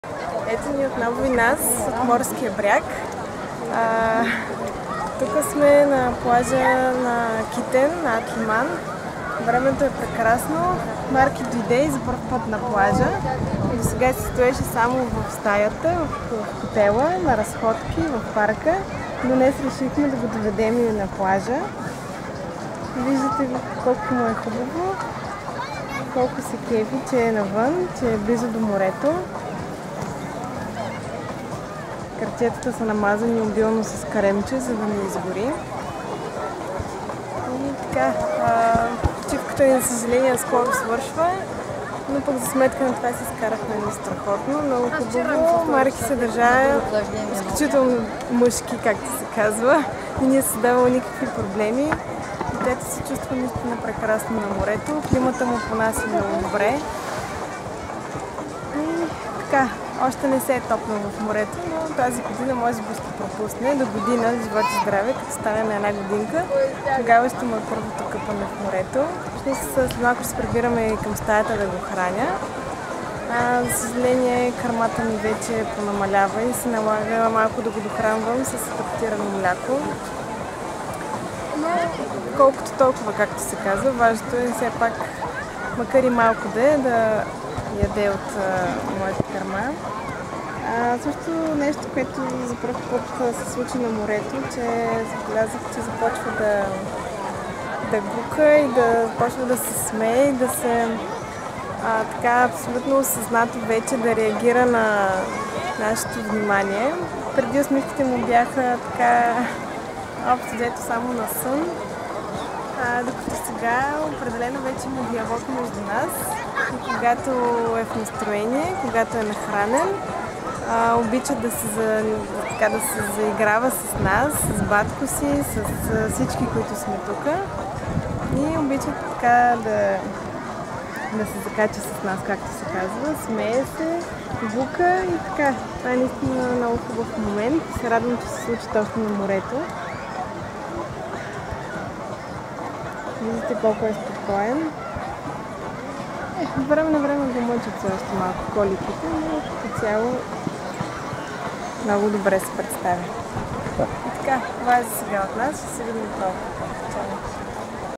Марк на море
Онлайн училище за родители - Dechko.bg, ви представя Марина и Марк, които ни изпращат морски поздрави.